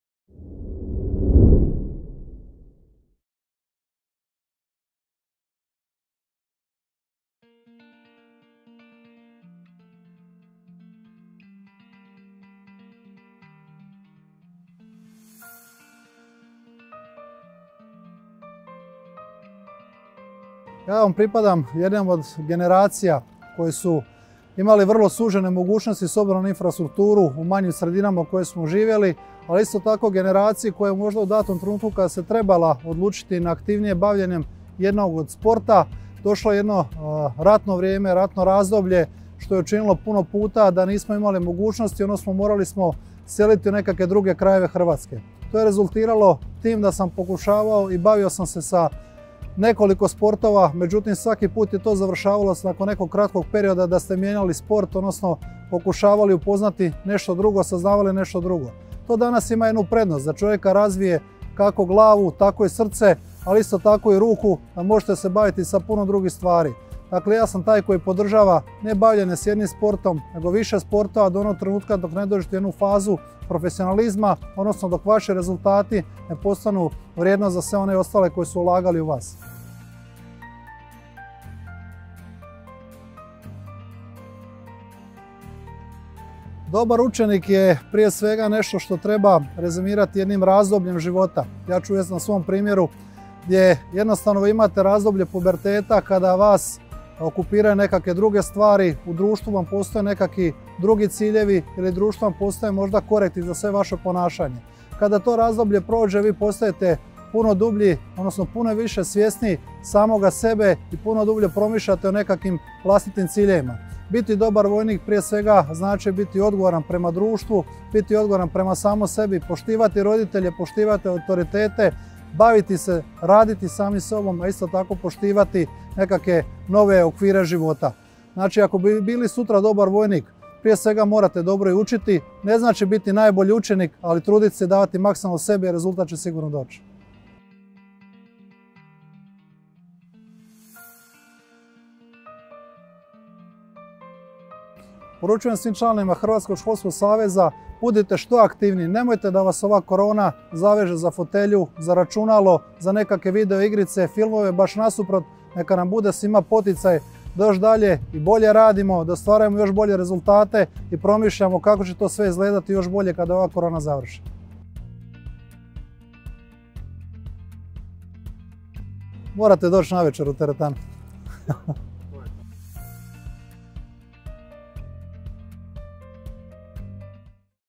ONLINE VJEŽBAONICA – MARIO BANOŽIĆ
U današnjoj Online vježbaonici gost nam je ministar obrane dr. sc. Mario Banožić. Ministar Banožić je kao vojnik uvijek morao biti spreman i u dobroj tjelesnoj kondiciji pa je tako s nama podijelio svoja iskustva iz sporta, ali i odlike dobrog vojnika. Poslušajte što nam je sve rekao ministar i ne zaboravite ostati tjelesno aktivni i zdravi!
Danas možete ponoviti naš prvi trening, a već u ponedjeljak slijedi novi, trening s našim ambasadorom, Mariom Mlinarićem.